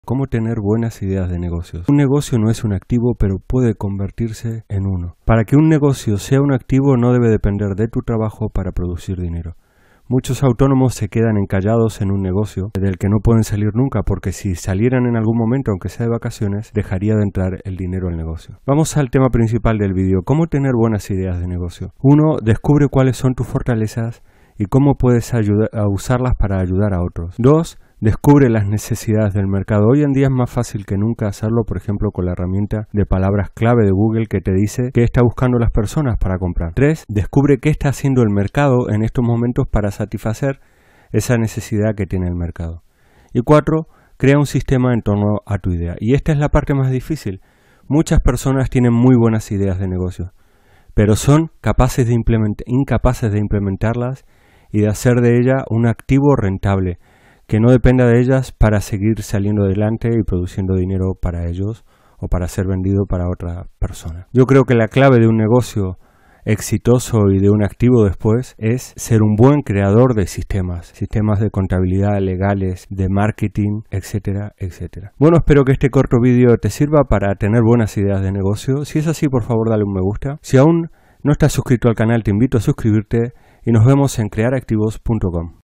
🤔¿Cómo tener buenas IDEAS de NEGOCIOS? [EMPRENDEDORES]😛
🎯¿Cómo tener buenas ideas de negocios? Esta es la pregunta que se hacen muchas personas que quieren un puesto de trabajo. En este video te damos más perspectiva sobre el tema. Vivimos en una sociedad donde se da culto a la idea pero se odia los procesos. Los procesos son más importantes que las ideas. Muchas ideas han muerto en los cerebros de sus creadores, porque ellos no conocían la forma de llevarlas a cabo.

Si tienes buenas ideas de negocios, mira este video. Porque también necesitas aprender a realizar procesos y sistemas que transforman tus ideas en realidad.

00:00 ¿Cómo tener ideas de negocios?
00:10 ¿Cómo convertir tu negocio en un activo?
00:30 ¿Cuales son tus fortalezas?
00:36 ¿Cuales son las necesidades del mercado?
00:46 ¿Cómo se satiface esa necesidad en este momento?
00:55 ¿Cómo crear sistemas entorno a las ideas para emprender?
01:13 ¿Cuál es la clave para tener un negocio éxitoso?
01:27 Conclusiones